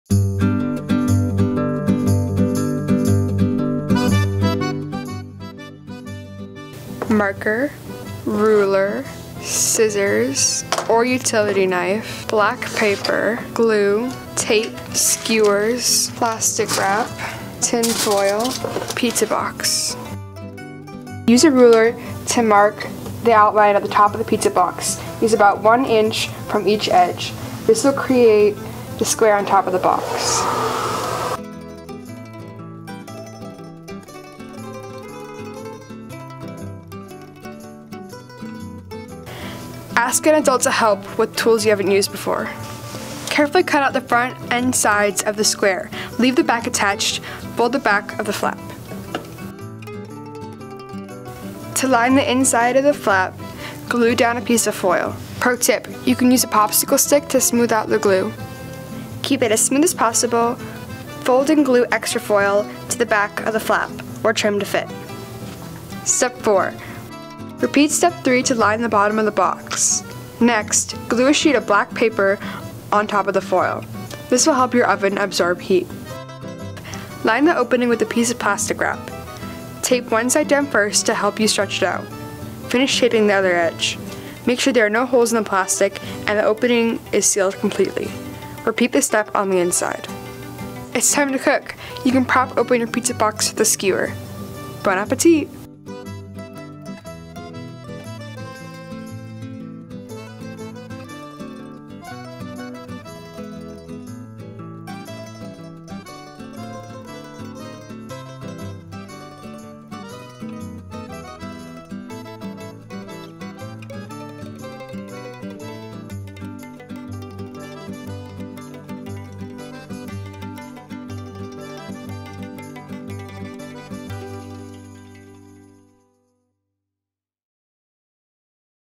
How to Make an Outdoor Solar Oven
Take outdoor cooking to the next level with this homemade solar cooker.

What you’ll need:

• Black paper
• Foil
• Glue or tape
• Large square pizza box
• Marker
• Plastic wrap
• Ruler
• Scissors
• Skewer